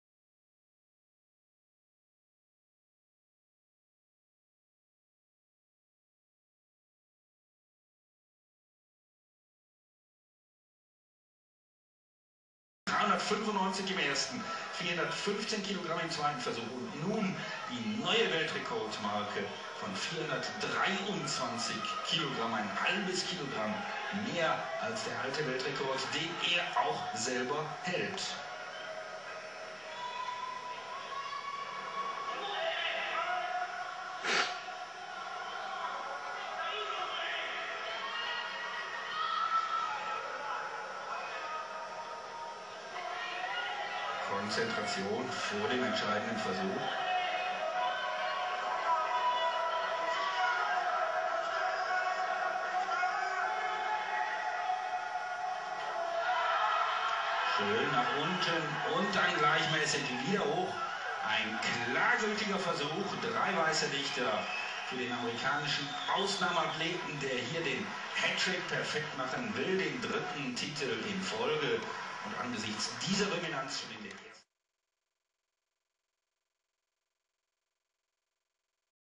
Ed Coan - 423@100 Unbreakable World Record
IPF World Championship 1994. Gold Medal
Squat - 423 kg (WR), bench press - 235 kg, deadlift 377.5 kg. Total - 1035 kg (WR)
Bodyweight - 99.4 kg